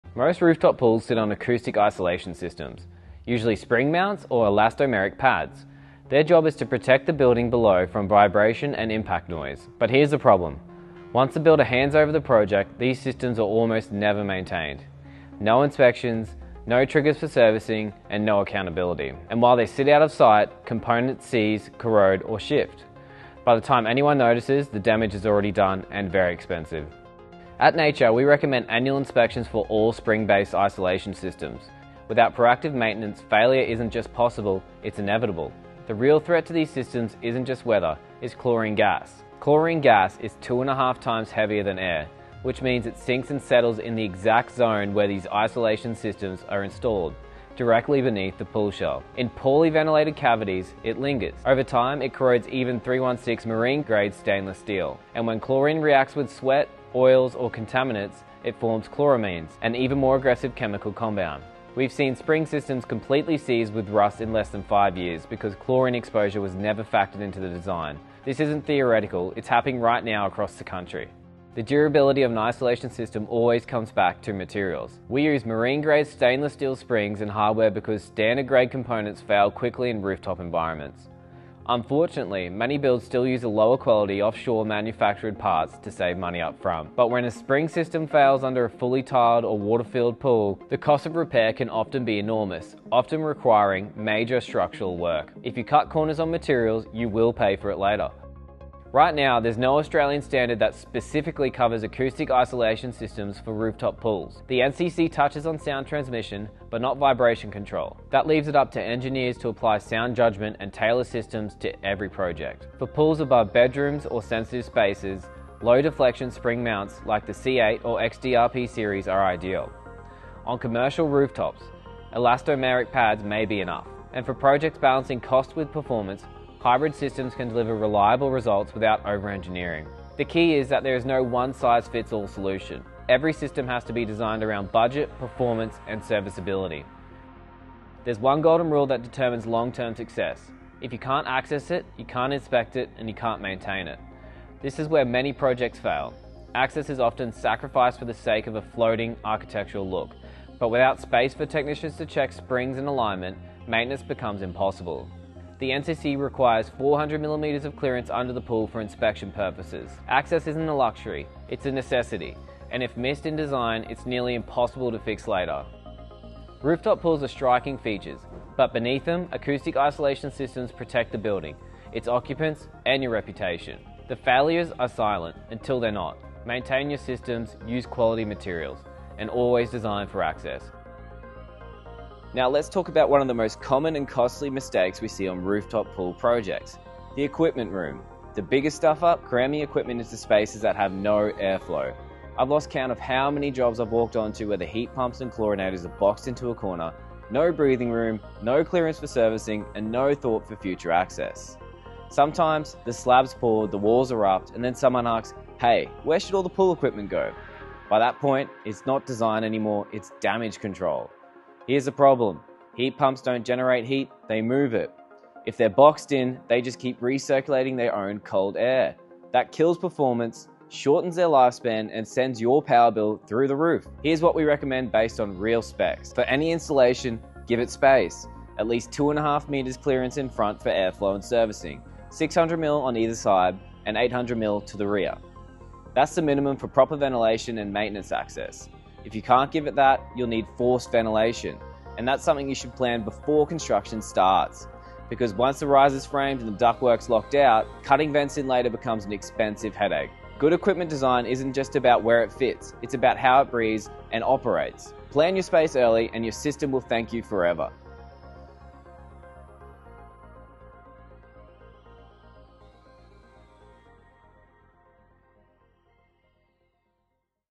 The Silent Threat Beneath Rooftop Pools
Most rooftop pools look incredible but what’s happening underneath them can be a completely different story.

In this video, I break down the hidden engineering behind rooftop pool acoustic isolation systems and the potential failures that almost no one talks about. From seized springs and corroded hardware to chlorine gas damage and poor equipment room design, these issues are happening right now across Australia.

You’ll learn:
• Why rooftop acoustic systems fail years earlier than expected
• How chlorine gas destroys even “marine-grade” stainless steel
• The real cost of using cheap offshore components
• Why access clearance is the #1 rule for long-term success
• How to design equipment rooms that actually work
• The airflow, servicing and ventilation requirements most projects ignore

Whether you’re a builder, architect, engineer, or developer, this video could save you hundreds of thousands in repairs  or prevent a major failure before it happens.

If you want your rooftop pool to last, you need to understand one thing:
The failures are silent until they’re not.